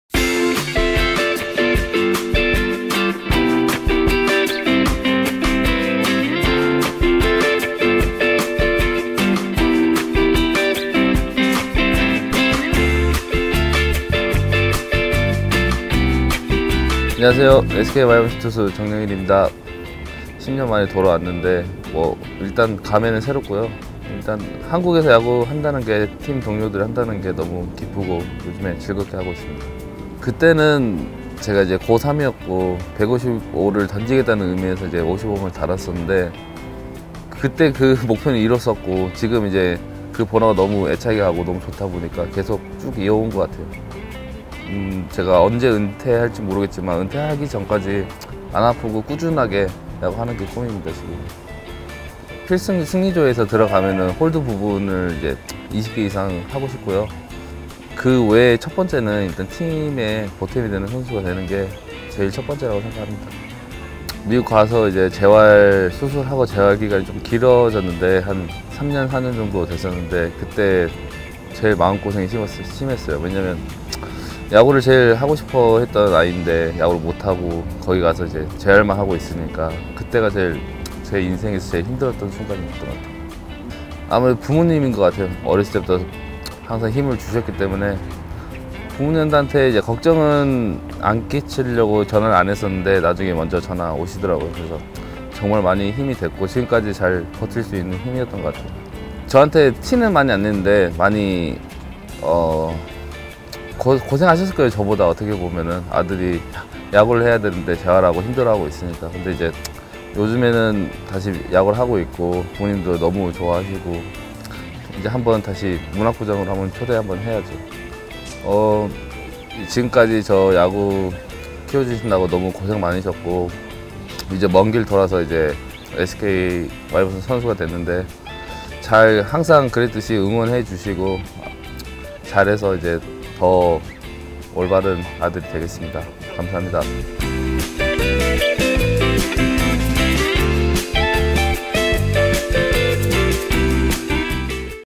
[SK 라이징 스타] No.55 정영일 1화
이번 라이징 스타의 주인공은 먼 길을 돌아 한국 무대로 컴백한 정영일 선수입니다.
지난 화요일, KIA와의 경기에서 1이닝을 12구 만에 삼자범퇴로 처리하며 가능성을 보여주었는데요.
정영일 선수의 야구 이야기, 그리고 부모님을 향한 진심어린 메시지를 만나보세요.